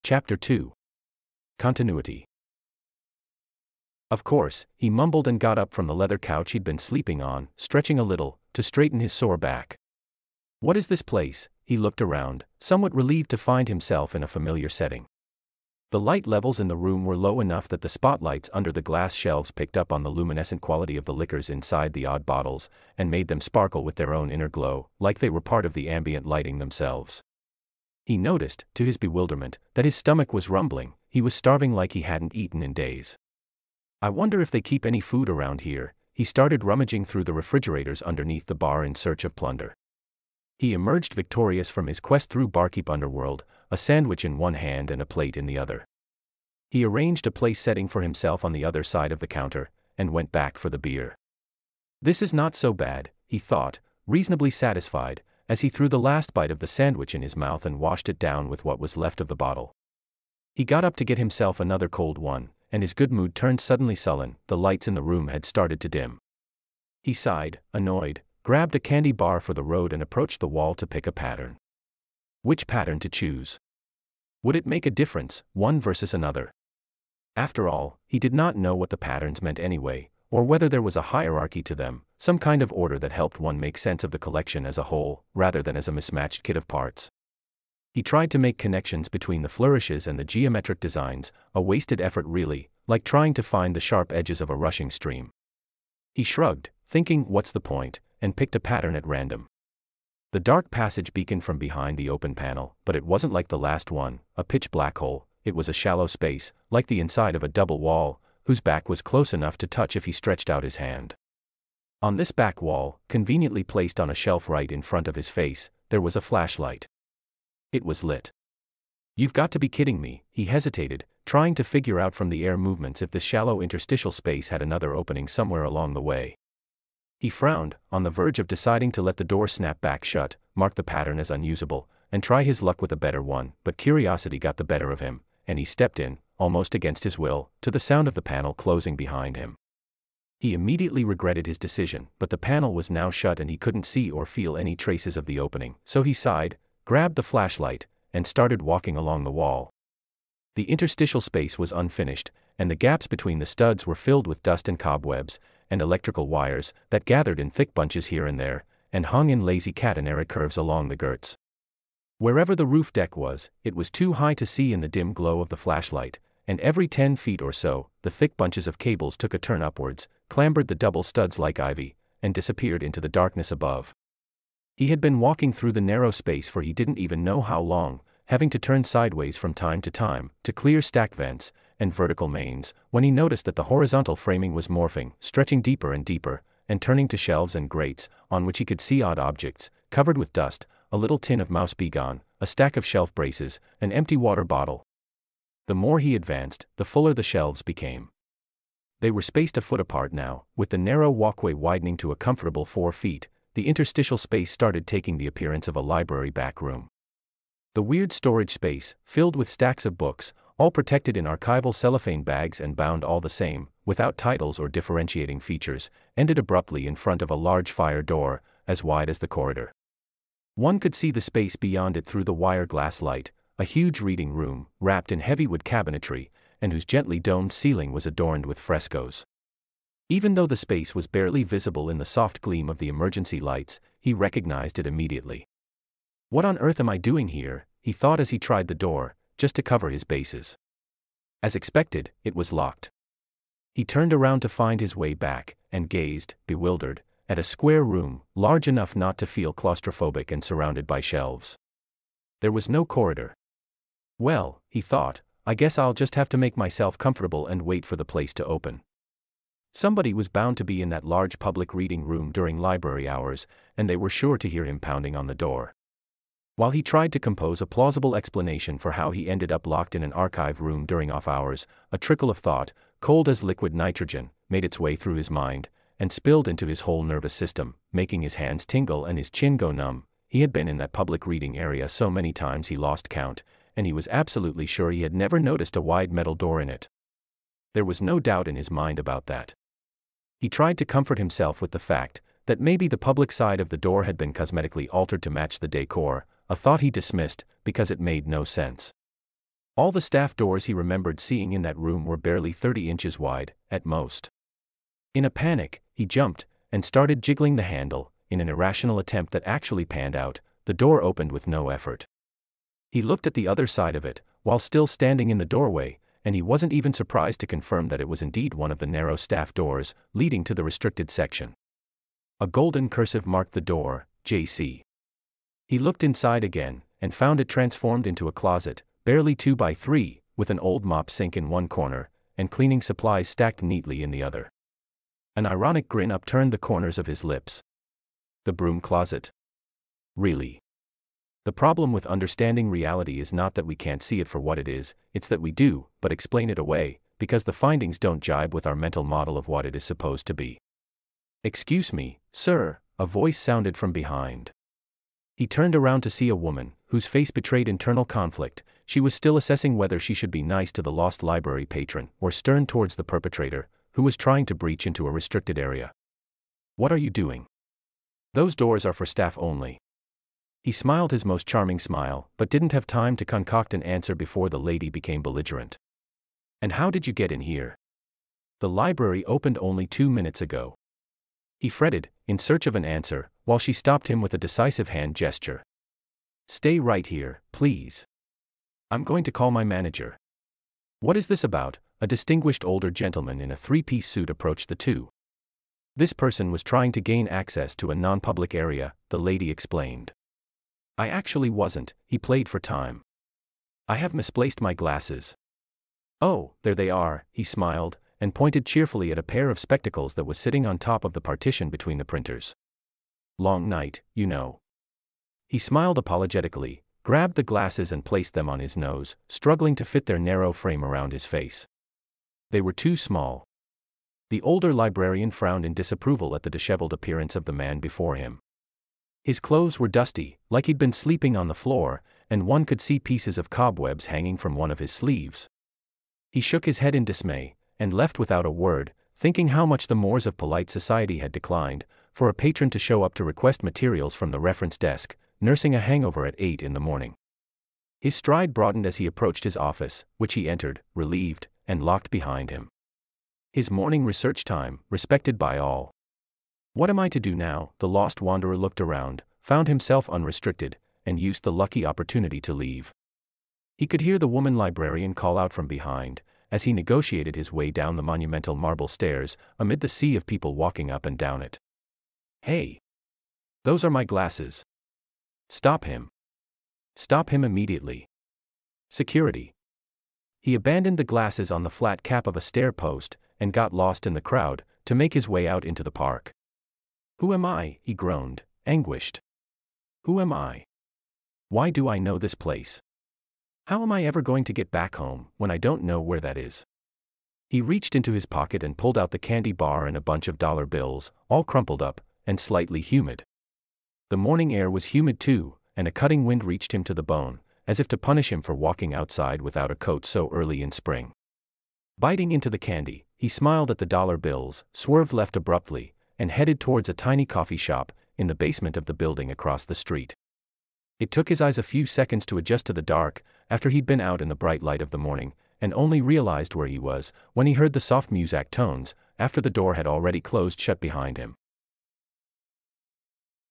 The Room In Between Chapter 2 - Continuity
In a panic, he jumped and started jiggling the handle in an irrational attempt that actually panned out: the door opened with no effort. He looked at the other side of it, while still standing in the doorway, and he wasn’t even surprised to confirm that it was indeed one of the narrow staff doors leading to the restricted section. A golden cursive marked the door JC. He looked inside again and found it transformed into a closet barely two by three, with an old mop sink in one corner and cleaning supplies stacked neatly in the other. An ironic grin upturned the corners of his lips.
“The broom closet? Really?”
The problem with understanding reality is not that we can’t see it for what it is, it’s that we do see it, but explain it away because the findings don’t jibe with our mental model of what it is supposed to be.